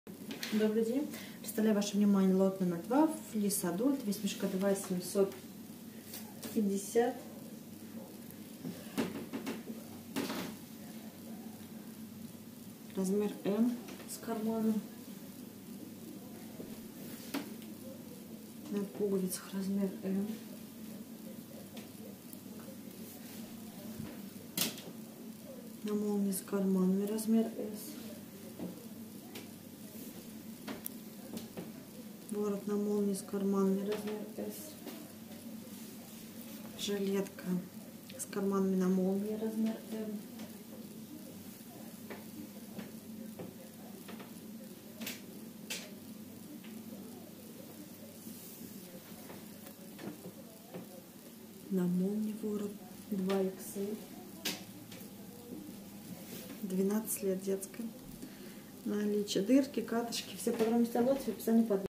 Fleece Adult 2,Цена за кг : 6.09 евро ,секонд хенд одeжда оптом
Fleece Adult (лот № 2)
Флисовые кофты мужские и женские (секонд хенд)
Страна: Англия
Сезон: Демисезон
Цена за кг.: 6.09 евро
Цена за мешок: 16.75 евро
Цена за ед.: 2.1 евро